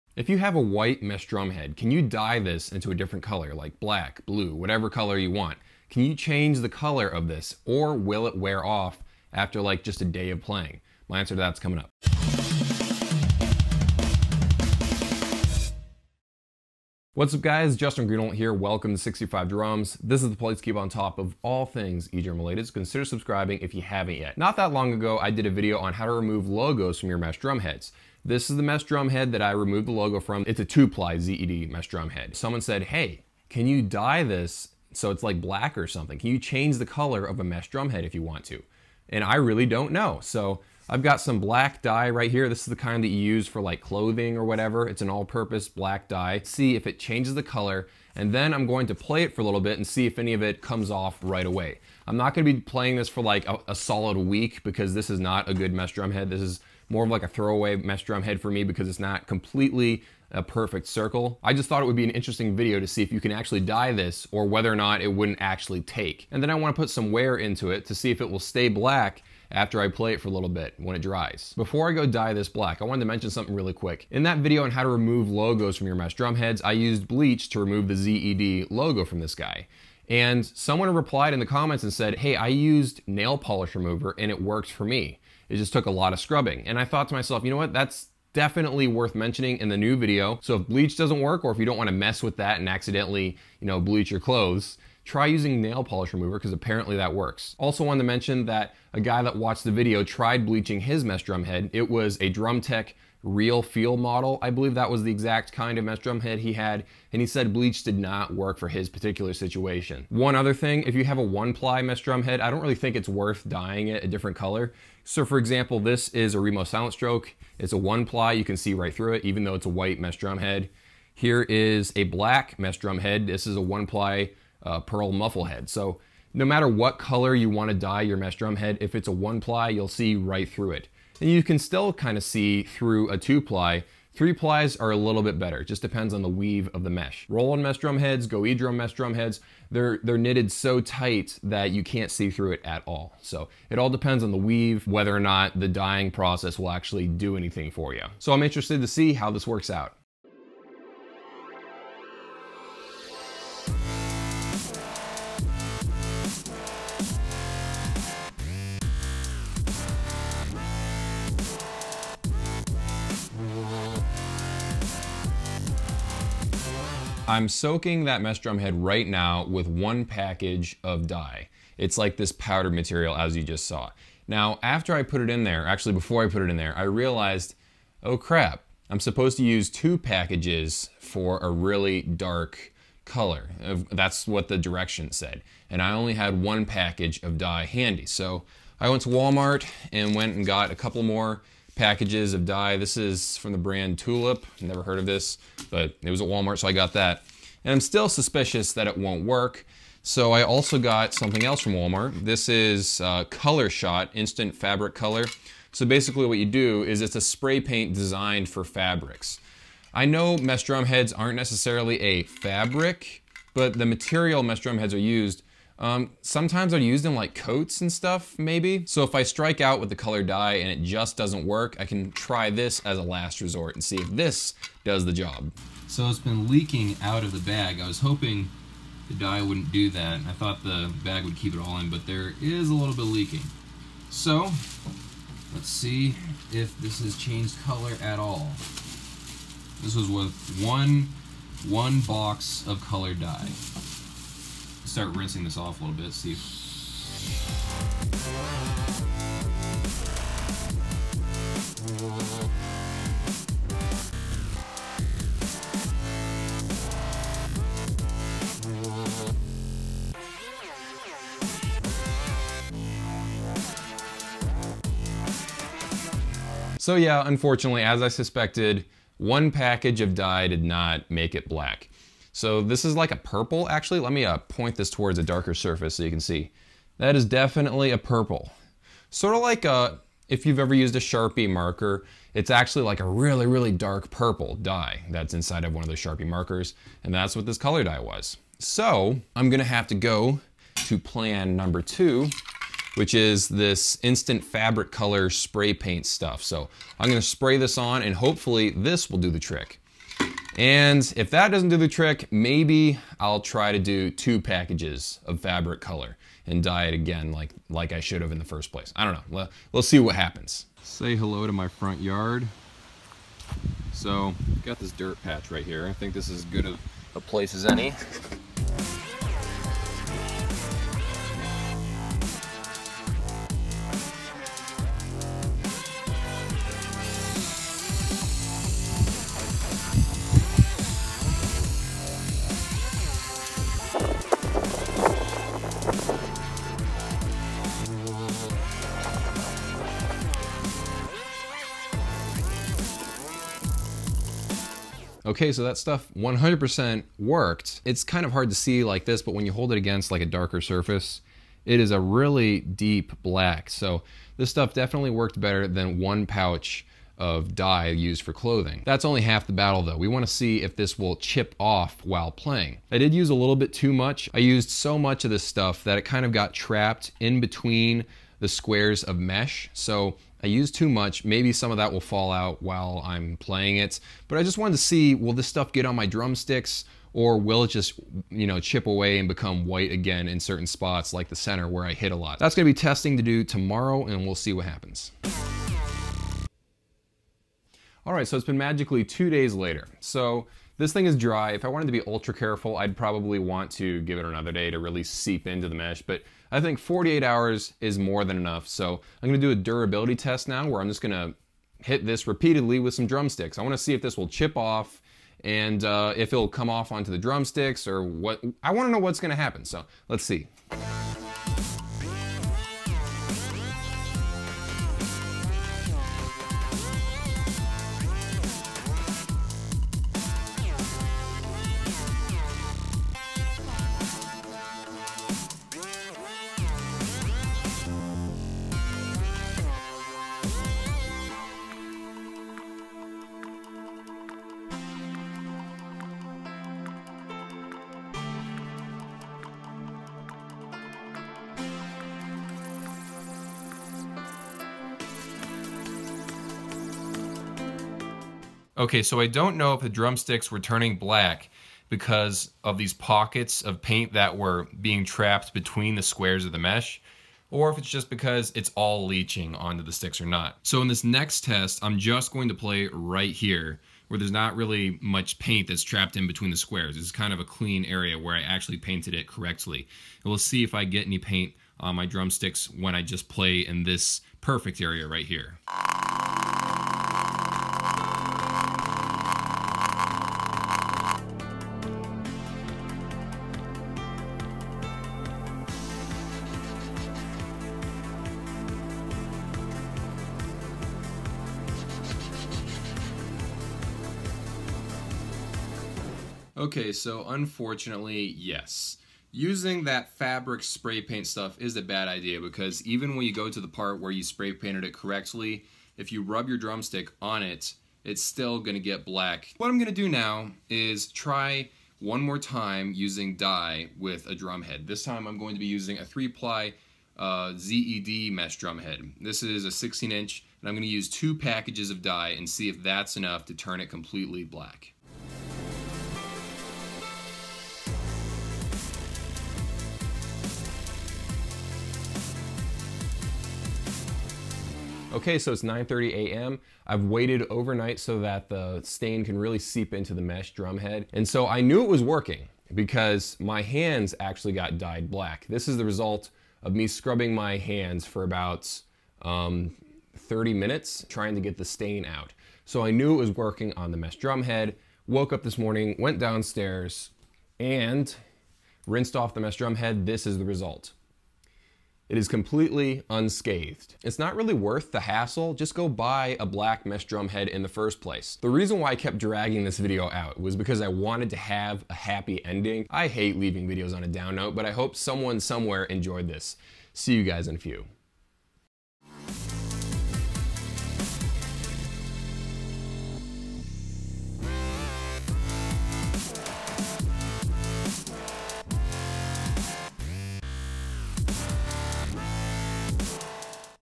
Can You Dye A White Mesh Head Black?
Is it possible to change a mesh drum-head to a different color? Black, Red, White? After a bunch of different tests I did over a few days, I've learned that it's not really worth the hassle. But soaking a mesh head in water with AS MANY bags of colored dye as you can get, is the best way.

65 Drums tshirt Store!

My Favorite Edrum Headphones:

My Favorite Drum Throne:

Handy Way To Record Your Electronic Drums: